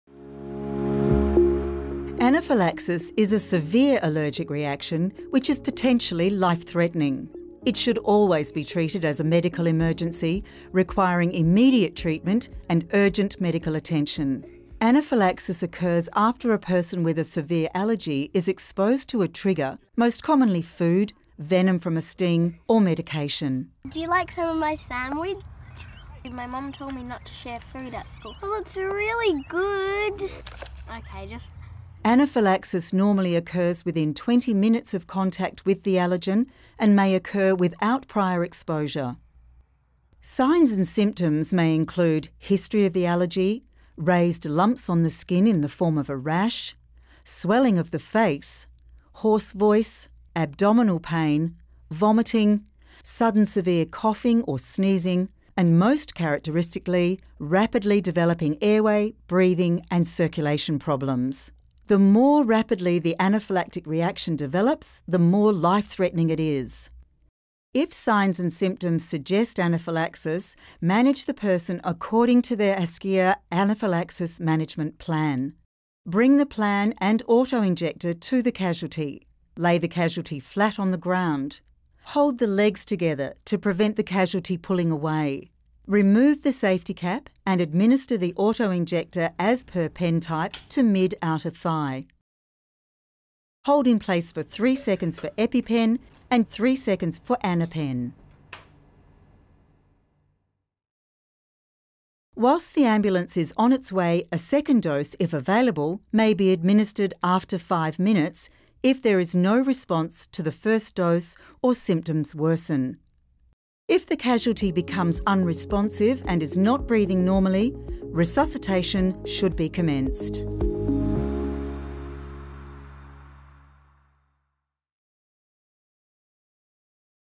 First Aid - Anaphylaxis
Anaphylaxis, or anaphylactic shock, is the most serious allergic reaction and can cause death without prompt medical attention. Within minutes of exposure to an allergen, 'or trigger', the person can have potentially life-threatening symptoms such as breathing difficulties.